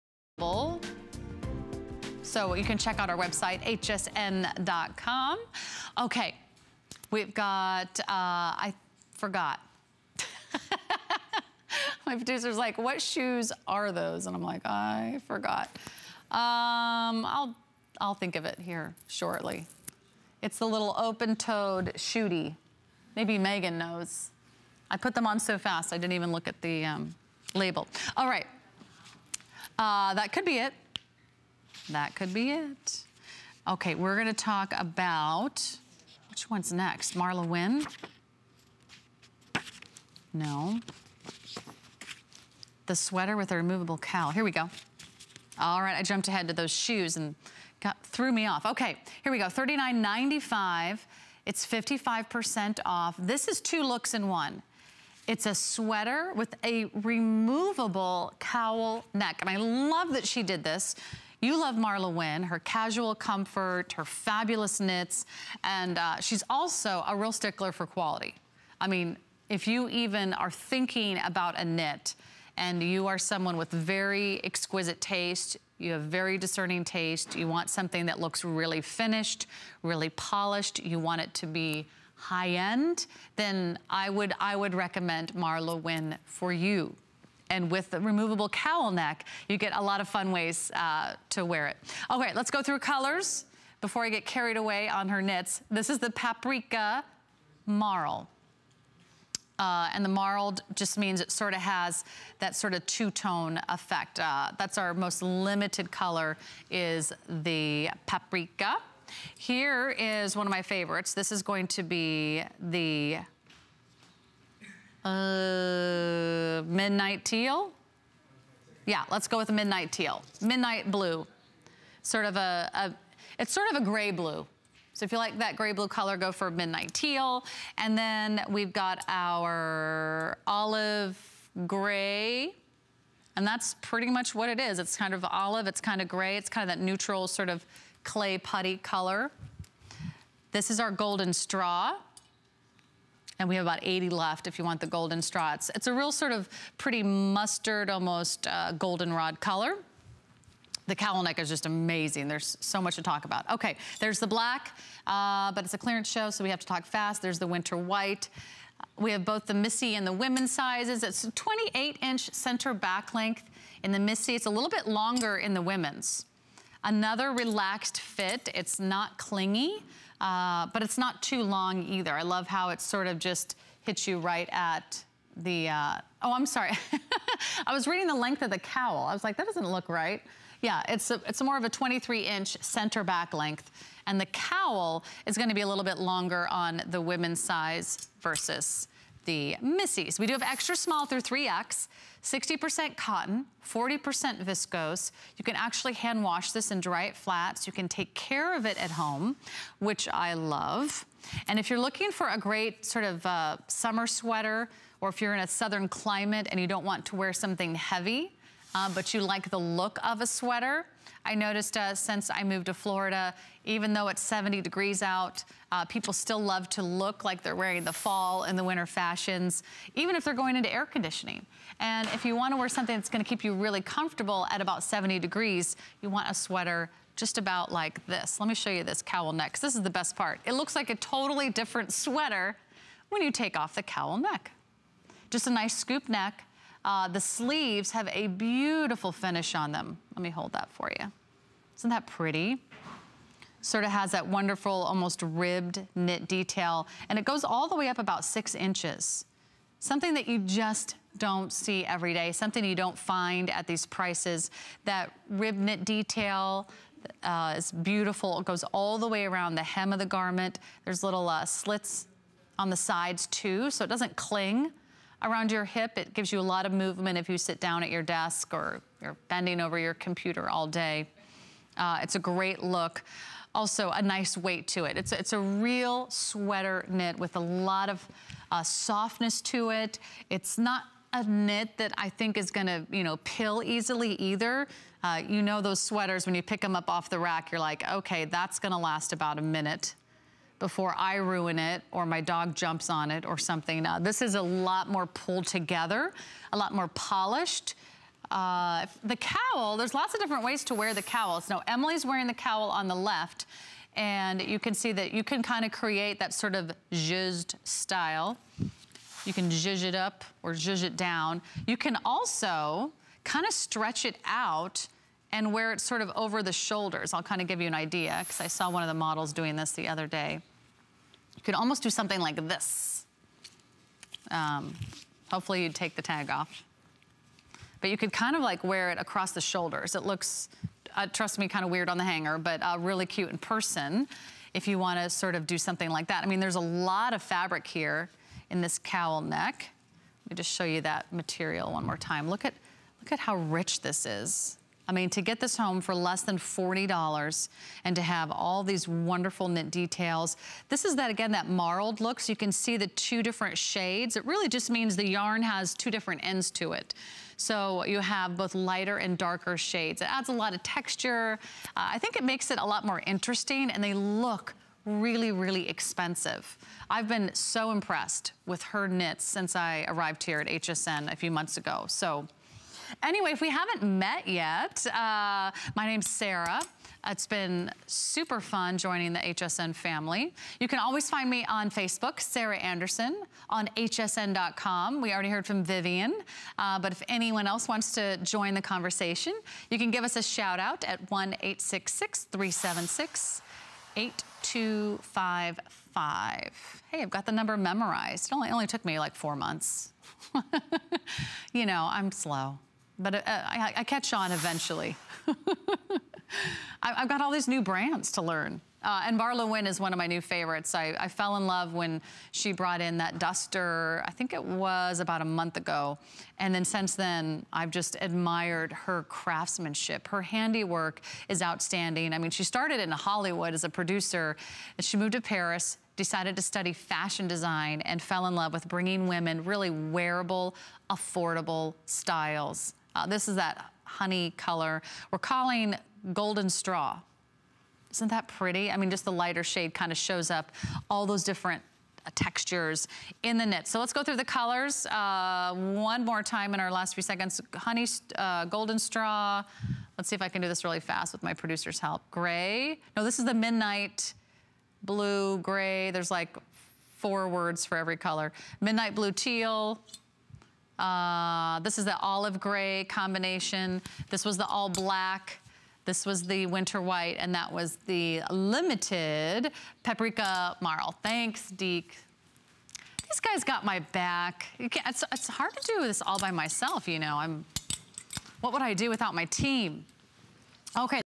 MarlaWynne Sweater with Removable Cowl
Step up your style game this season in a sweater that's sweeter with a cowl. This 2piece set gives you plenty of range, comfort and warmth. Onward and...